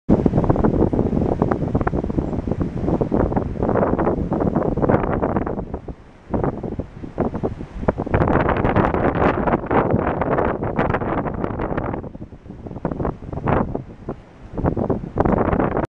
Bird vs Shell
A Bird trying to pick up a shell at Waldport Beach, Oregon